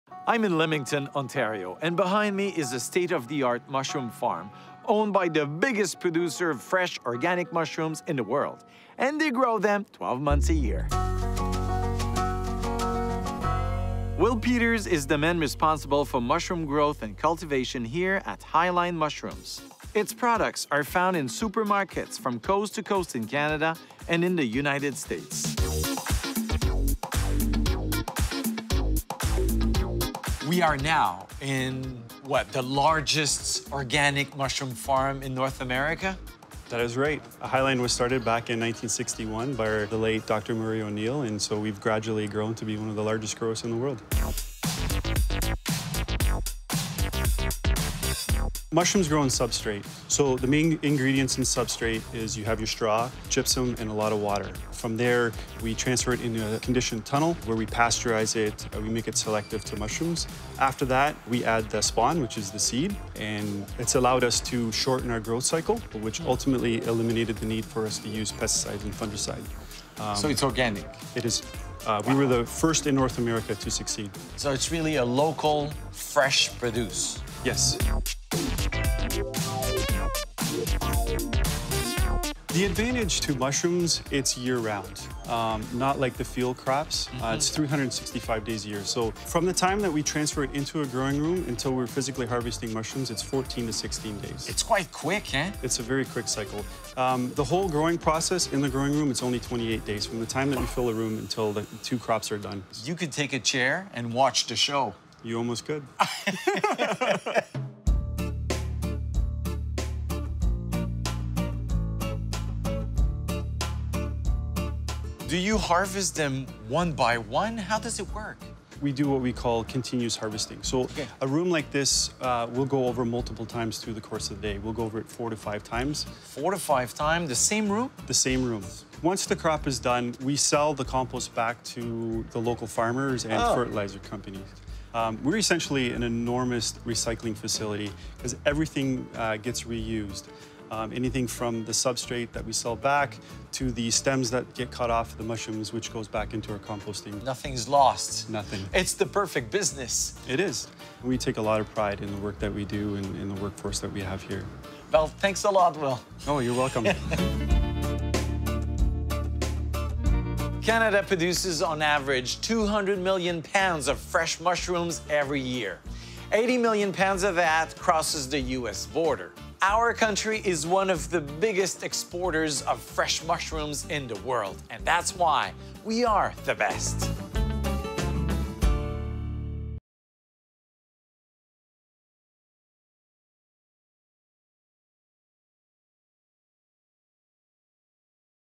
Take a tour of the largest organic mushroom farm in Canada | We Are The Best
Did you know Canada produces an average 200 million pounds of fresh mushrooms every year? We are one of biggest exporters of fresh mushrooms on the planet.

Will Peters is in charge of production at Highline Mushrooms, the largest organic mushroom farm in North America. At Highline, Peters and his team grown the mushrooms 365 days a year, without the use of pesticides and fungicide. Join chef Ricardo Larrivée as he tours Highline and learns just what it takes to become a top exporter.

CBC presents Canada 2017, a yearlong exploration of Canada and what it means to be Canadian as we mark 150 years since Confederation.

Take a tour of the largest organic mushroom farm in Canada